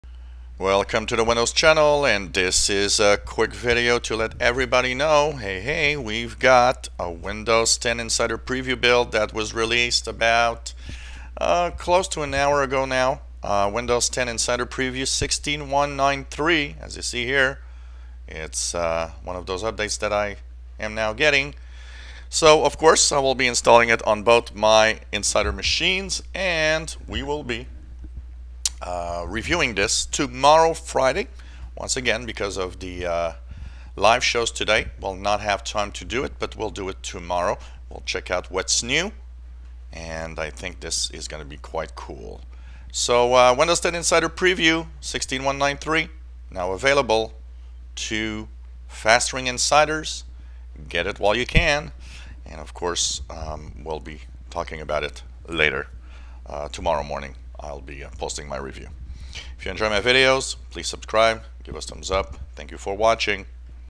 Windows 10 insider preview build 16193 Released for Fast Ring May 11th 2017
Another build of Redstone 3 is available for the fast ring 16193